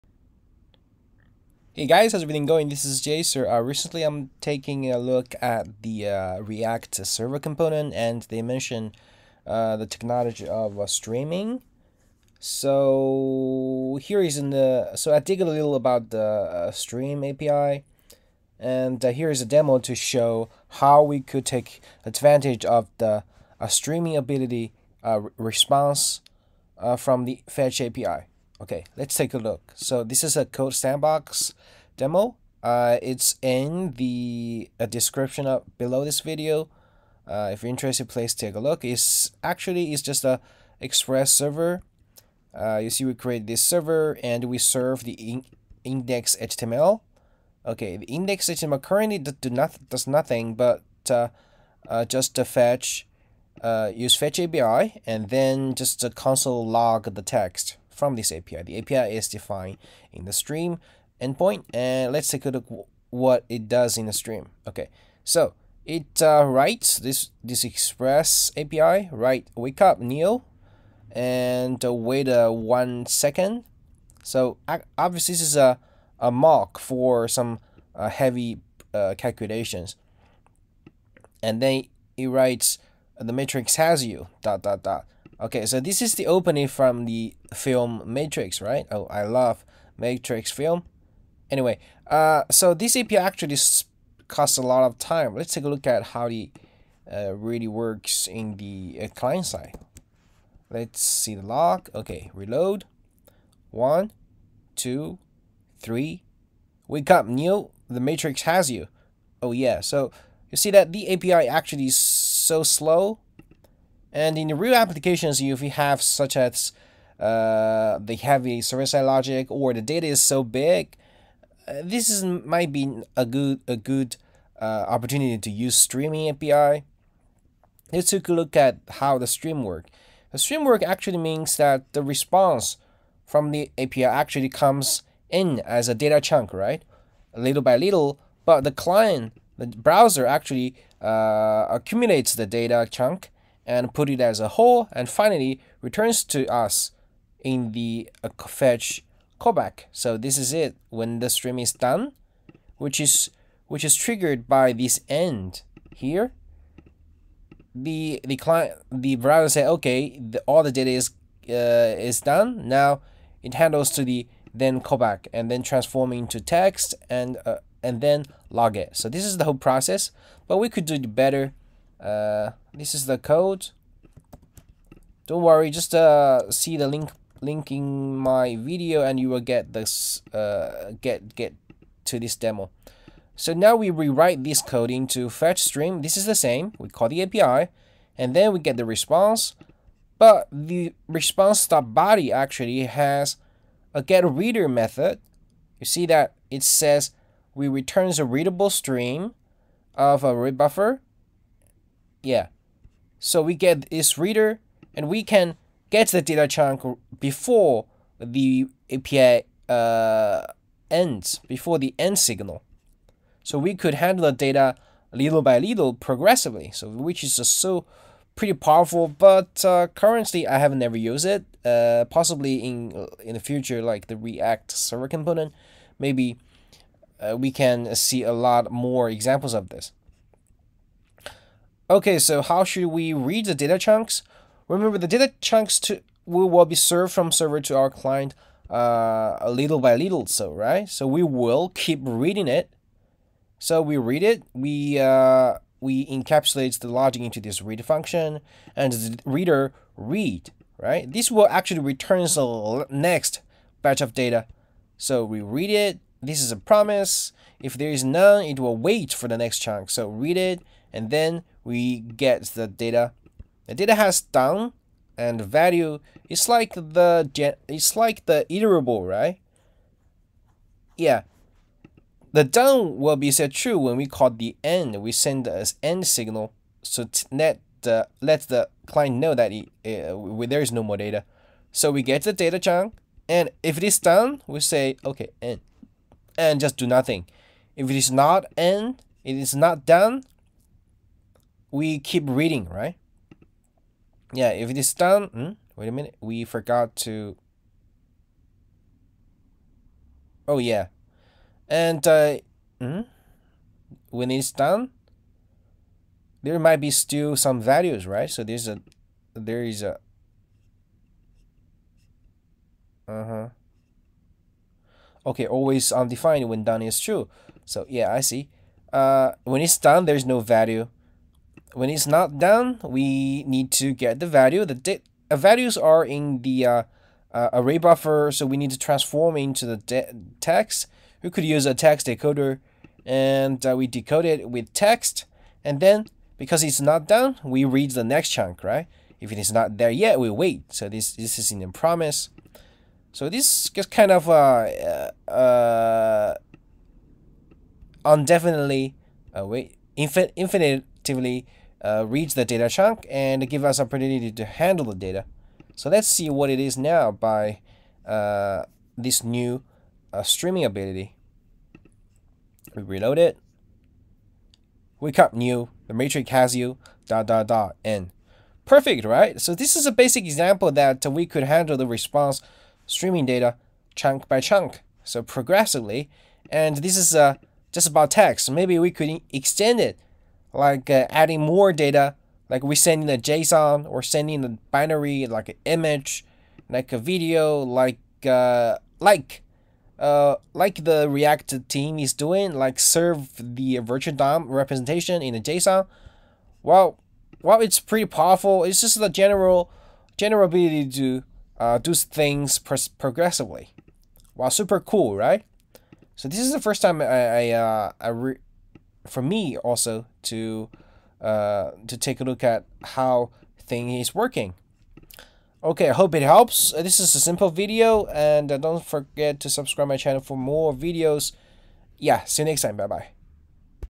[Code Demo] Using Response Stream from Fetch API | JSer - Front-End Interview questions
Fetch API Response Stream gives us the possibility to handle response in a progressive way rather than waiting for the whole data to arrive. Pretty powerful if the server logic is heavy, or the response is purely too much a burden.

and currently practicing leetCode & BFE.dev. If you are interested, maybe we can learn together.